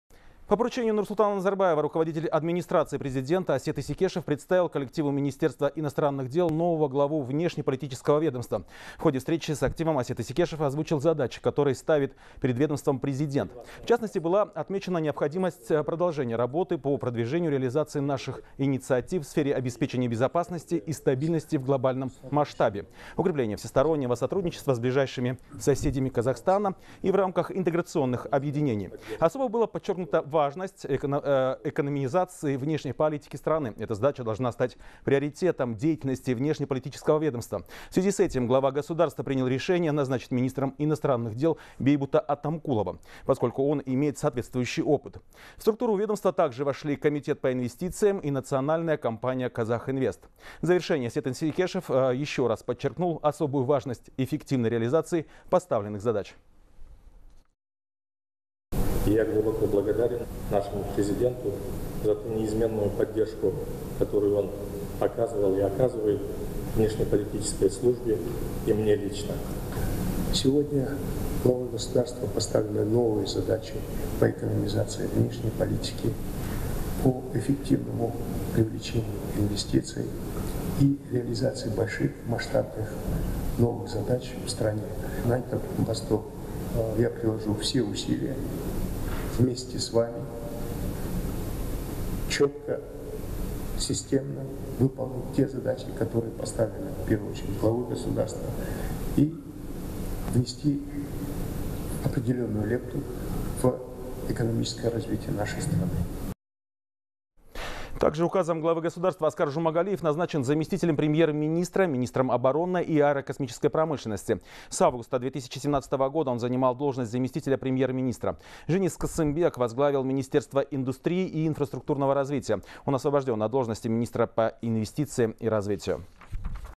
А.Исекешев представил коллективу МИД нового руководителя
По поручению Нурсултана Назарбаева руководитель Администрации Президента Асет Исекешев представил коллективу Министерства иностранных дел нового главу внешнеполитического ведомства.

В ходе встречи с активом Асет Исекешев озвучил задачи, которые ставит перед ведомством Президент. В частности, была отмечена необходимость продолжения работы по продвижению реализации наших инициатив в сфере обеспечения безопасности и стабильности в глобальном масштабе, укрепления всестороннего сотрудничества с ближайшими соседями Казахстана и в рамках интеграционных объединений.